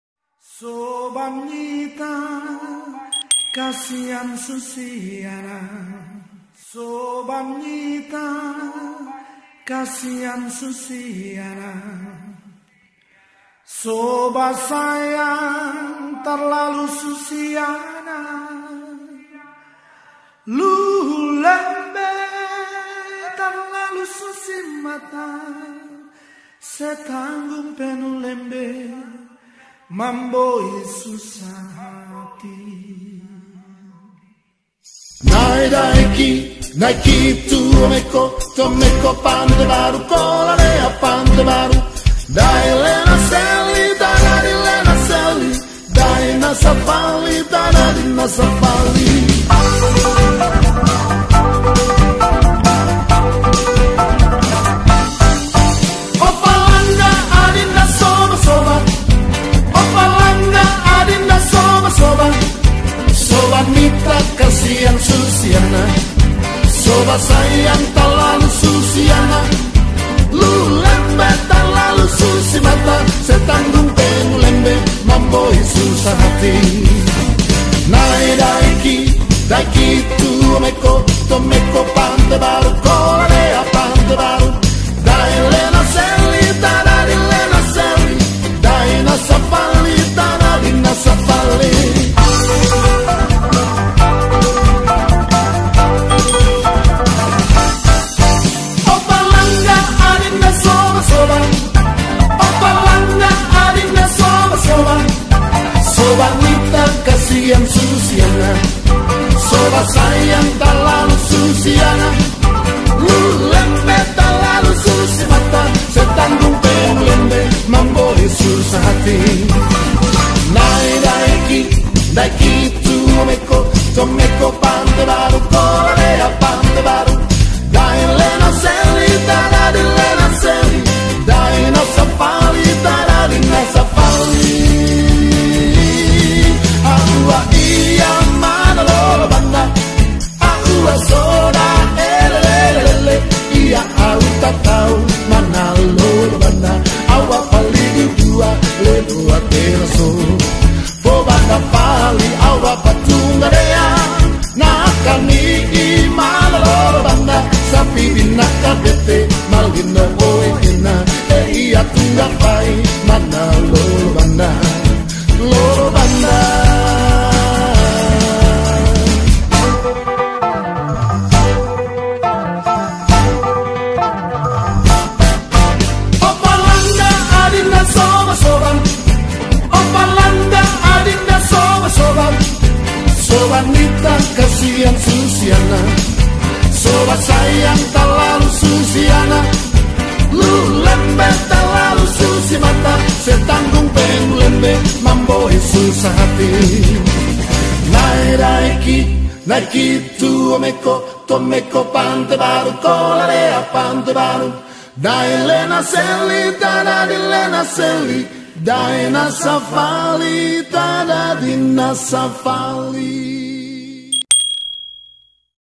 TARI OFALANGGA | 2020 BY. SMPN 4 KUPANG
Tarian OFALANGGA ini merupakan materi UJIAN AKHIR SEMESTER GANJIL, Ujian praktek Tari pada mata pelajaran Seni Budaya SMP kelas VII pada lembaga pendidikan SMP NEGERI 4 KUPANG.
Peserta didik mengikuti proses pembelajaran BDR secara online dari rumah selama masa COVID-19.

Terima Kasih👼🙏